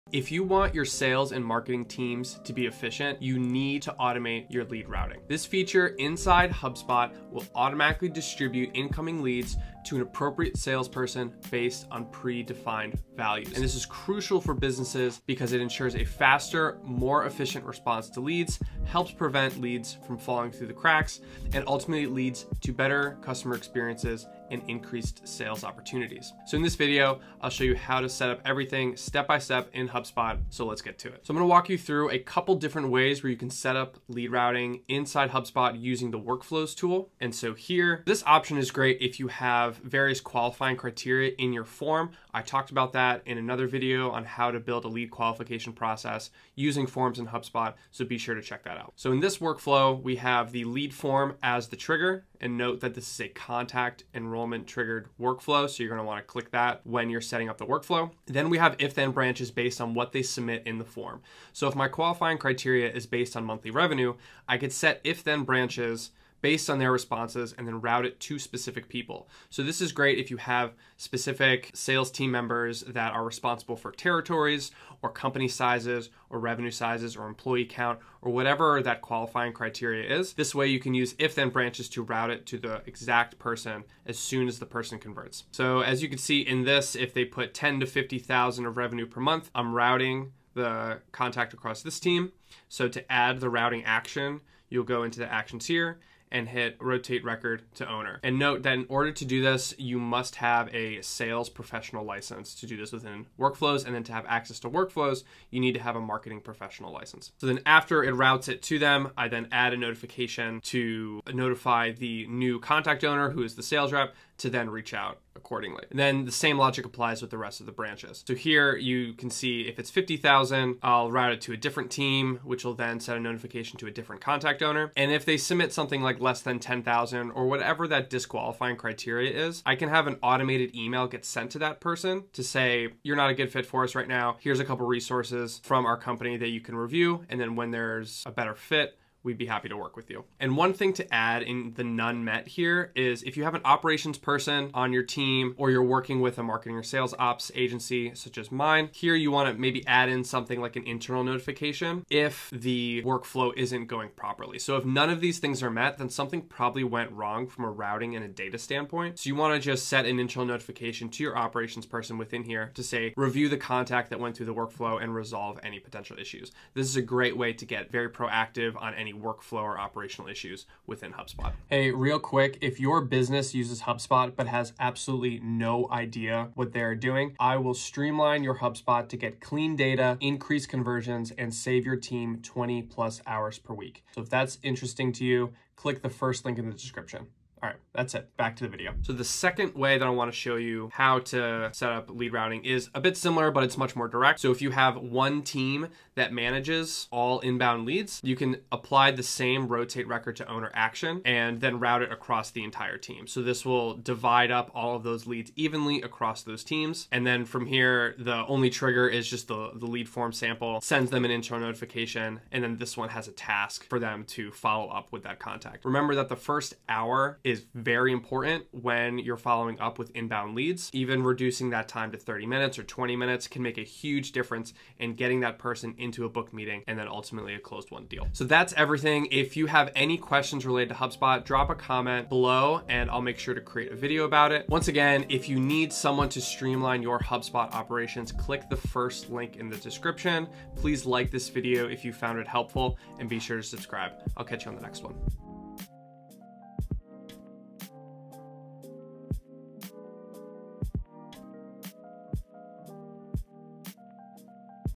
How to Automate Lead Routing Inside HubSpot (Step-By-Step Tutorial)
Want someone to manage and streamline your HubSpot to get clean data, increase conversions, and save your team 20+ hours/week?